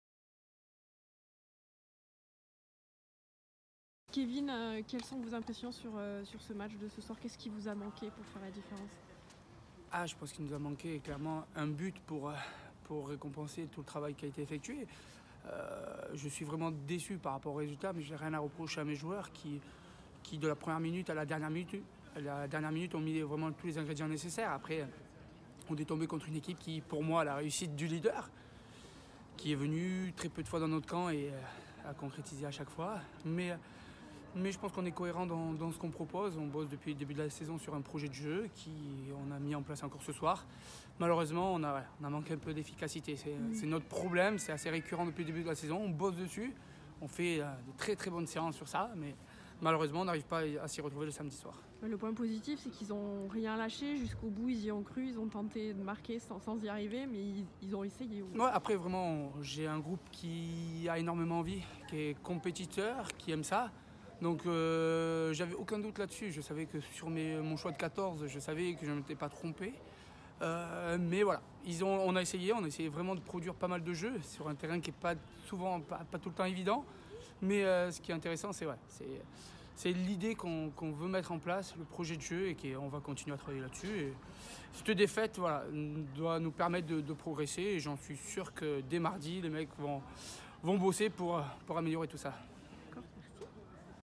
La réaction de Kévin Pirs (Le Bouscat, R2)
Le Bouscat - FC Mascaret : 0-2
Stade des écus, Le Bouscat
Journée 8 (match en retard) de régional 2 poule D